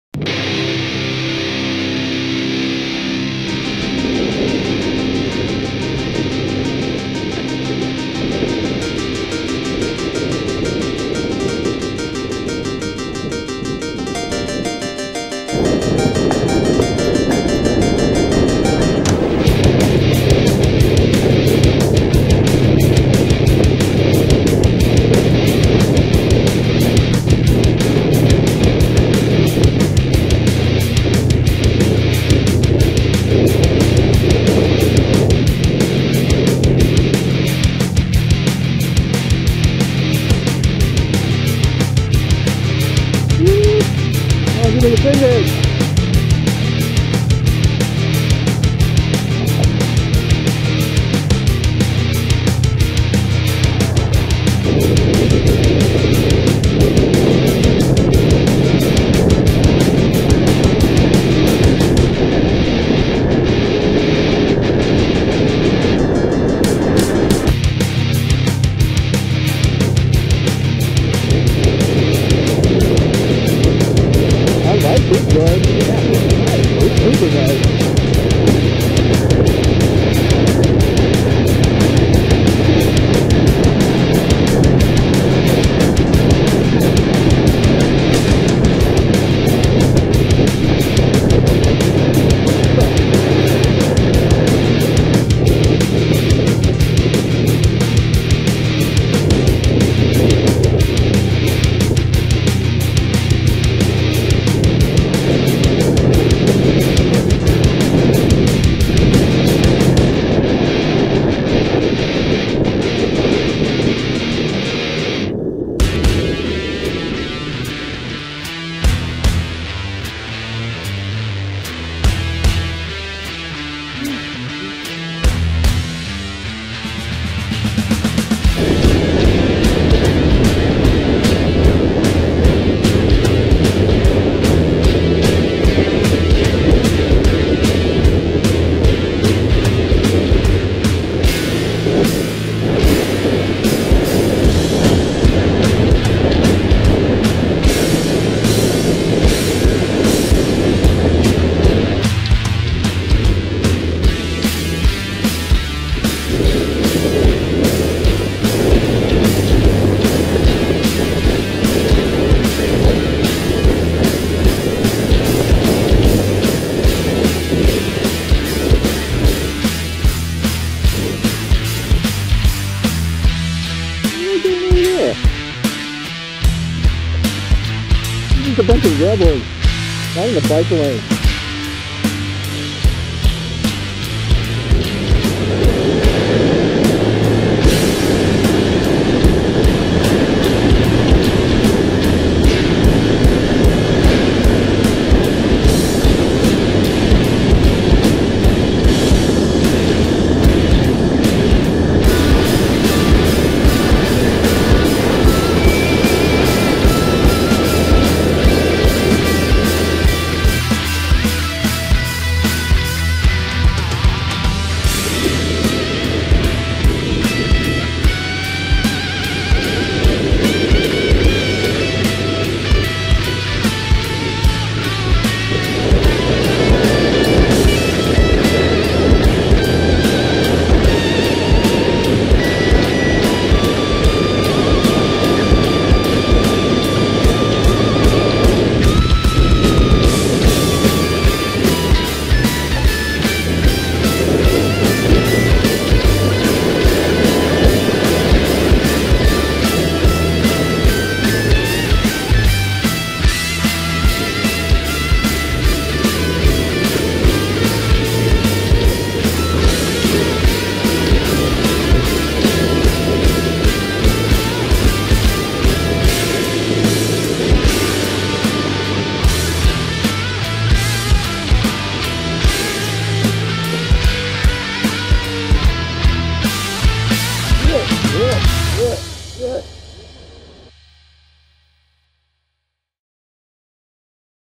Electric Unicycle Atlanta Westside Beltline Tour - Electric Wheelers of Georgia
A nice glide and ride with members of EWOG and ATLesk8 along some of the Atlanta Beltline and downtown.